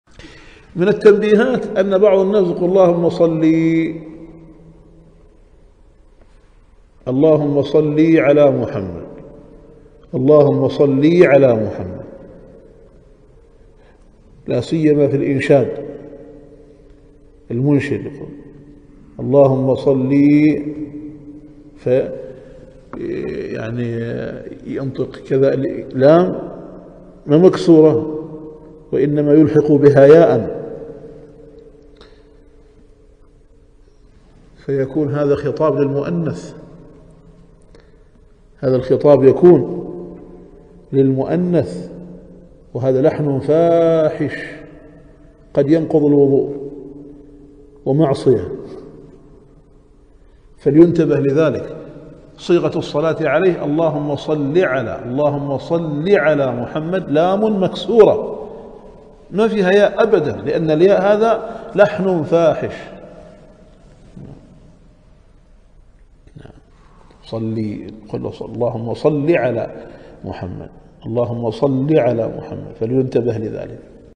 إحذر اللحن الفاحشن في اللهم ( صلي ) على محمد ! الشيخ مسعود المقبالي
مصدر المقطع:

إتحاف الأحياء بشرح الإحياء ... درس فضل الصلاة على النبي محمد صل الله عليه وسلم.